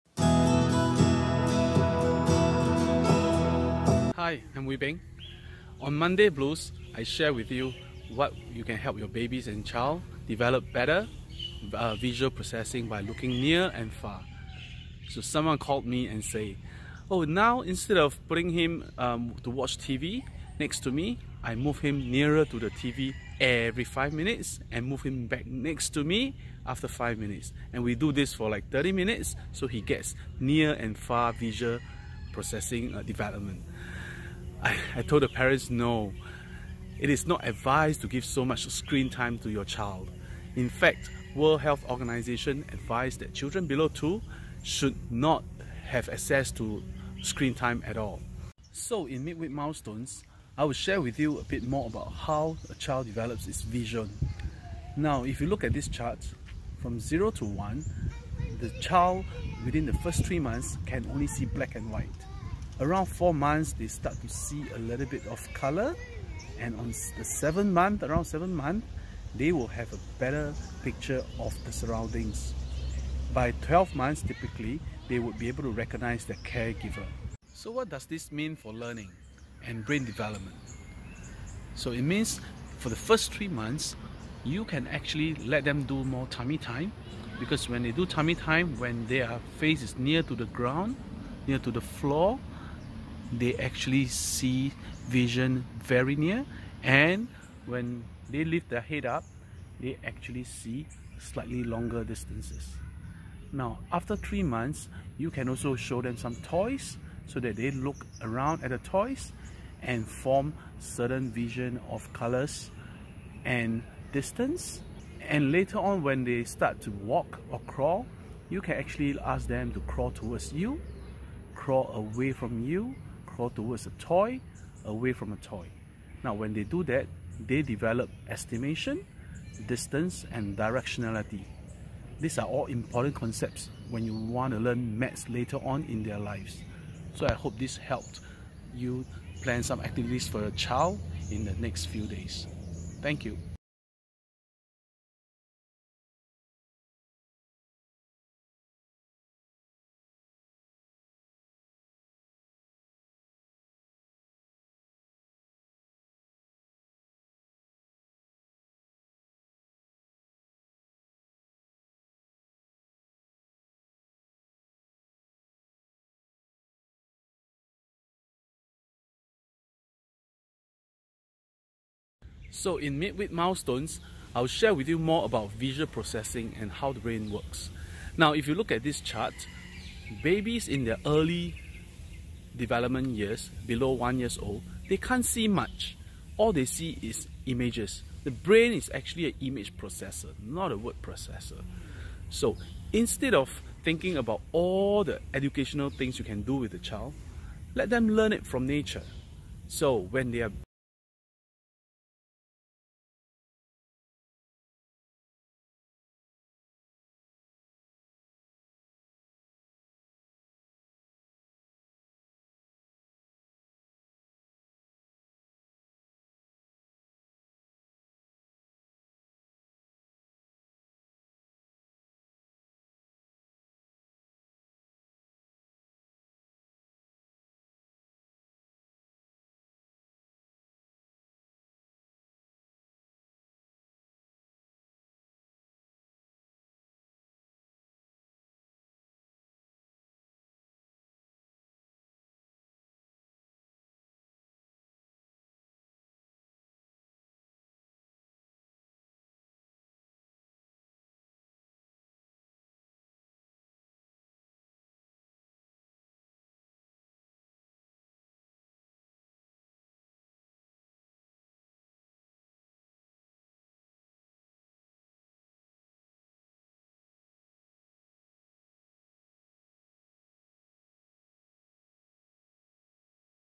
Stop watching educational videos during Lockdown! Visual development can be done naturally.
Stop watching educational video during Lockdown! Visual development can be done naturally by doing some scientifically proven activities at home.
Learn what you can do at home with your child during this period without compromising their brain development.